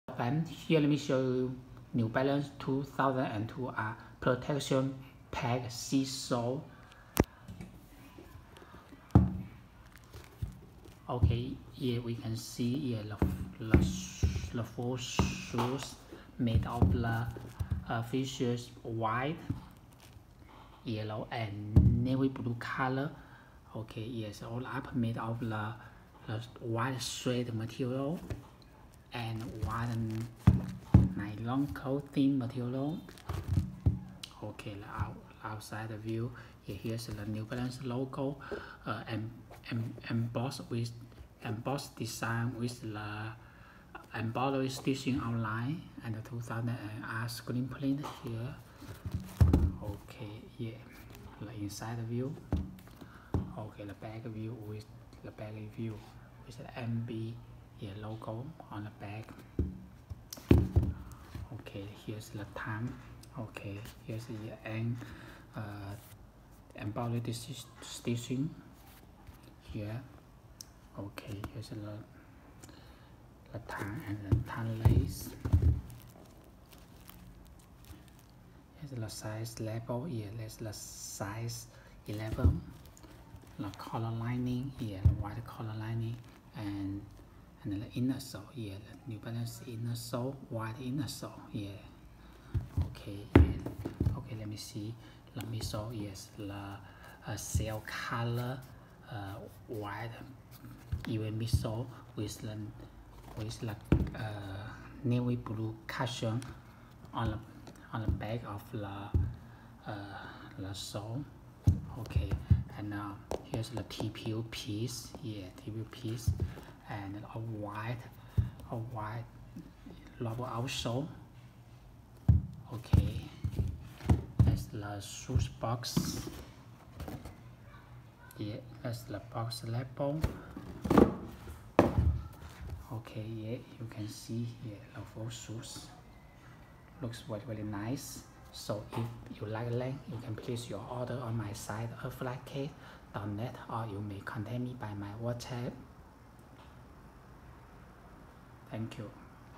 New Balance 2002R Protection Pack Sea Salt Review
New Balance 2002R Protection Pack Sea Salt

Hey friend today give you the 1st and detailed review on New Balance 2002R Protection Pack Sea Salt,if you like them, pls click the below link to order on my

The New Balance 2002R ‘Sea Salt’ is taken from the three-piece ‘Protection Pack,’ conceived by New Balance designer Yue Wu as a visual representation of sneakers that last a lifetime. The upper is crafted from white leather with eroded suede overlays throughout the forefoot, quarter panel and heel. Details include a tonal ‘N’ logo, exposed foam tongue and 2002R branding in red and blue. The sneaker rides on a distressed ABZORB midsole, supported underfoot by a durable N-ergy outsole.